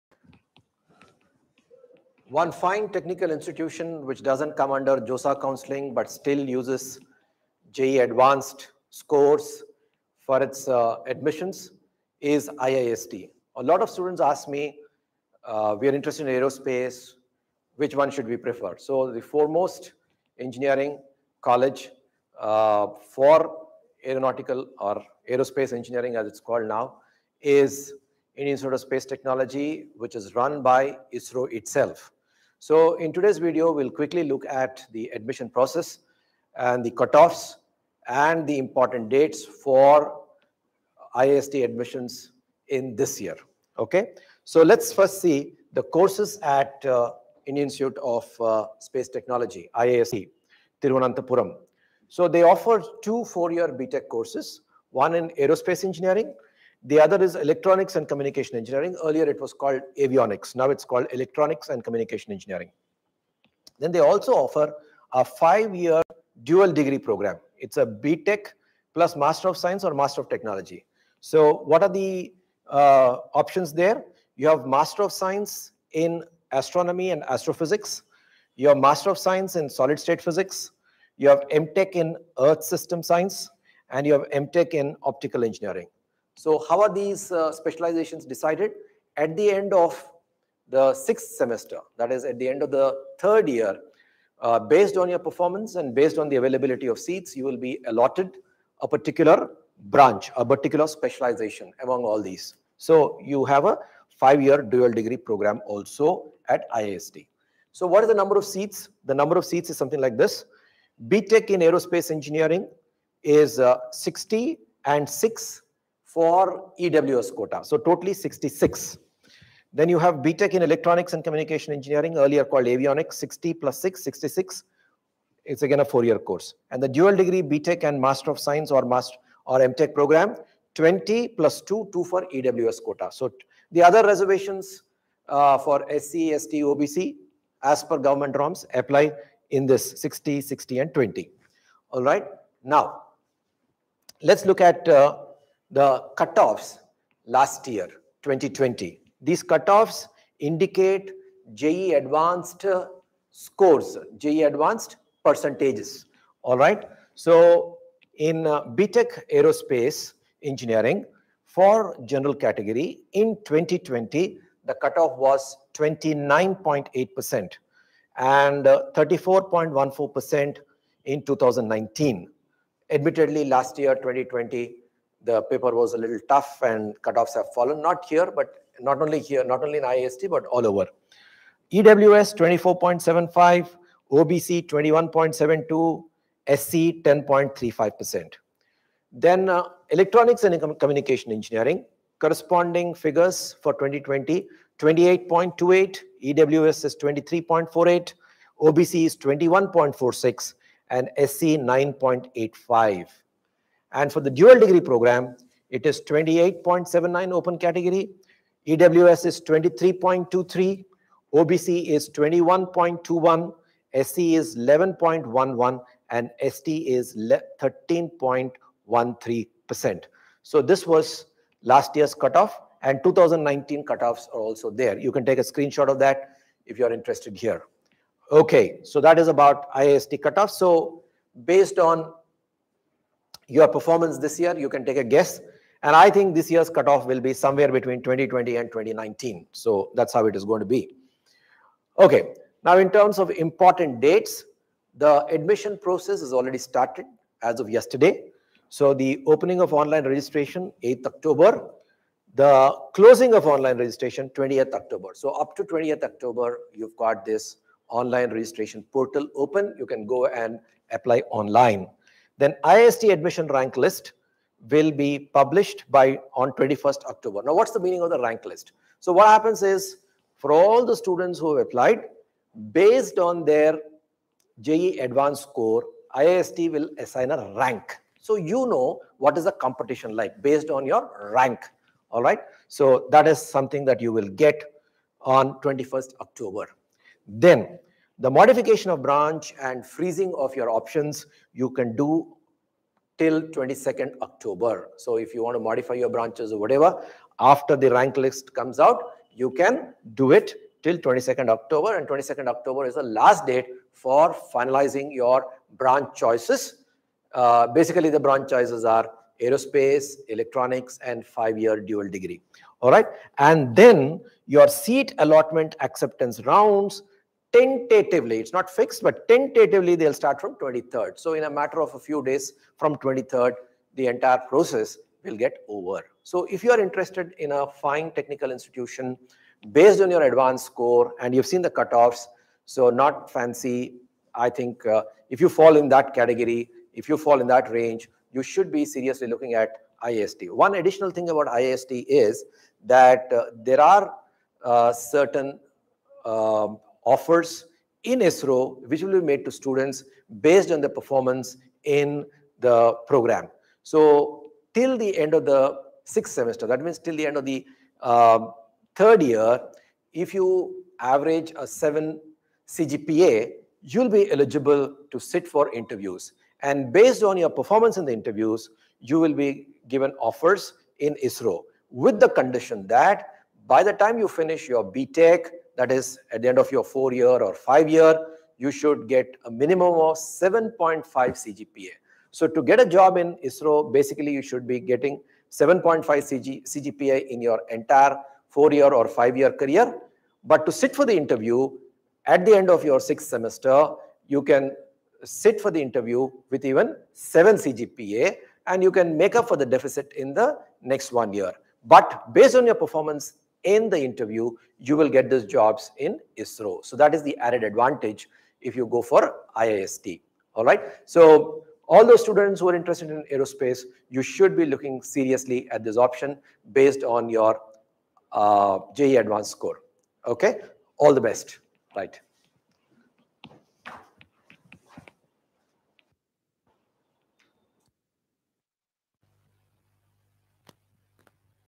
IIST Admissions 2021 II Counselling, Courses, Seat Matrix I Important Dates, Cut Offs I NANO Academy
IIST – Indian Institute of Space Technology, Thiruvananthapuram admissions 2021, Counselling process, Seat Matrix, Cutoffs in 2020 & 2019 based on JEE Advanced Marks, Important dates. IIST offers two 4 year BTech Courses & and a five year dual degree course. Based on performance in the course students are given job oofers in ISRO. IIST is not part of JOSAA counselling.
Courses Offered by IIST
4-Year B.Tech. ( Aerospace Engineering )
4-Year B.Tech. (Electronics and Communication Engineering (Avionics))
5-Year Dual Degree (B.Tech. + Master of Science/Master of Technology)
The Dual Degree Programme is a 5-year (10 Semesters) Programme. On successful completion, the students are awarded a B.Tech. degree in Engineering Physics and a Master of Science/ M.Tech. degree in one of the following four postgraduate streams:
Master of Science in Astronomy and Astrophysics
Master of Science in Solid State Physics
M.Tech. in Earth System Science
M.Tech. in Optical Engineering
Admission to UG Programmes is based on +2 and JEE Advanced 2021 for the
Important Dates
Opening of Online Registration and Filling-in of Branch Preferences October 8, 2021, 10:00 Hr
Closing of Online Registration October 20, 2021, 15:00 Hr
Publication of IIST Admission Rank List October 21, 2021, 17:00 Hr
Closing date for Modification of Branch Preferences October 22, 2021, 17:00 Hr
Seat Allotment/Acceptance rounds is tentatively scheduled to start from October 23, 2021
Reservations:
As per the reservation policy of Government of India applicable to Central Educational Institutions (CEI), candidates belonging to the following categories are admitted to reserved seats based on relaxed criteria. The categories and the corresponding percentage of reservation are
Scheduled Castes (SC): 15%
Scheduled Tribes (ST): 7.5%
Other Backward Classes (OBC) belonging to Non-Creamy Layer (NCL): 27%
Persons with Disabilities (PD) : 5% (Horizontal Reservation)
Economically Weaker Section (EWS)